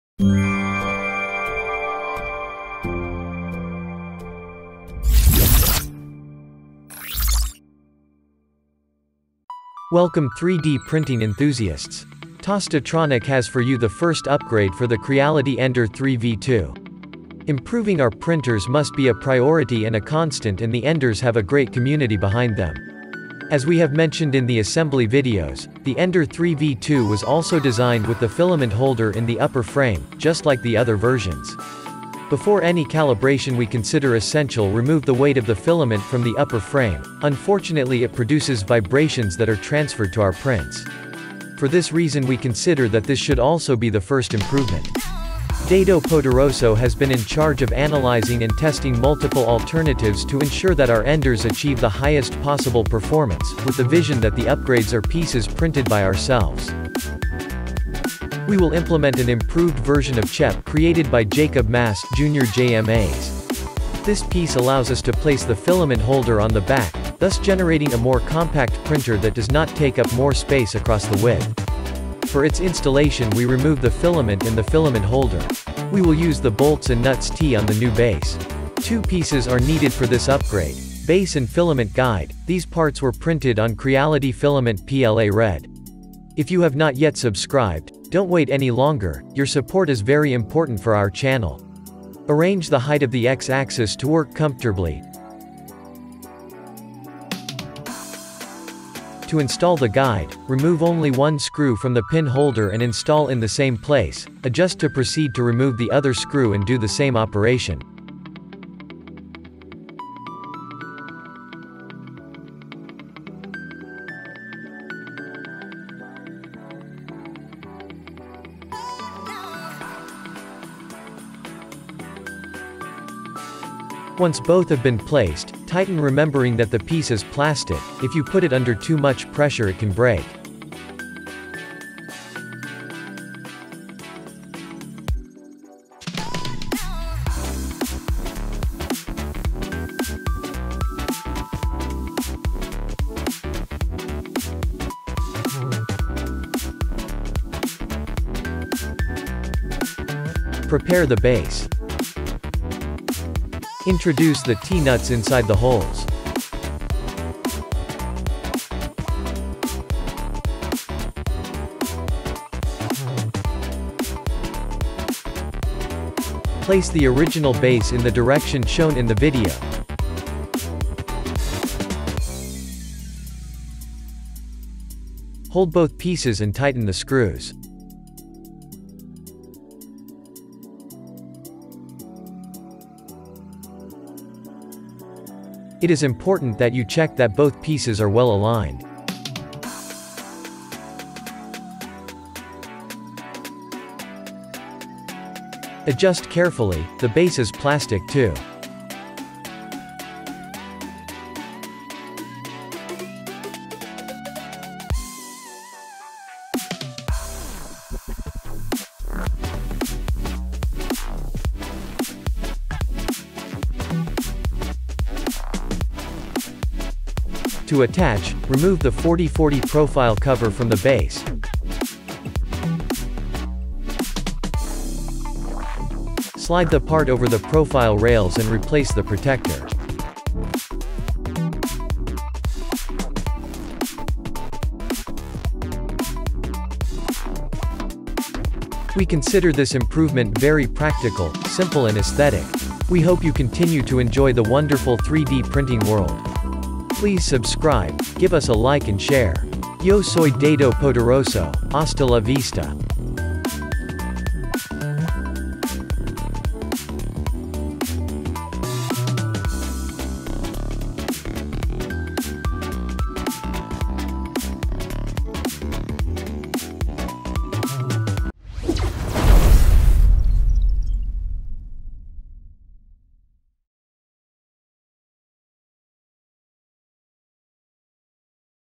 Ender 3 v2 - First Upgrade Filament Spool Holder - by Tostatronic
Visit our gallery of models and improvements for your 3d printers.

Tostatronic has for you the first upgrade for the Creality Ender 3v2.

Improving our printers must be a priority and a constant and the Enders have a great community behind them.

As we have mentioned in the assembly videos, the ender 3 v2 was also designed with the filament holder in the upper frame, just like the other versions.

Before any calibration we consider essential remove the weight of the filament from the upper frame, unfortunately it produces vibrations that are transferred to our prints. For this reason we consider that this should also be the first improvement.

We will implement an improved version of Chep created by Jacob Mass (Jr Jmas). This piece allows us to place the filament holder on the back, thus generating a more compact printer that does not take up more space across the width.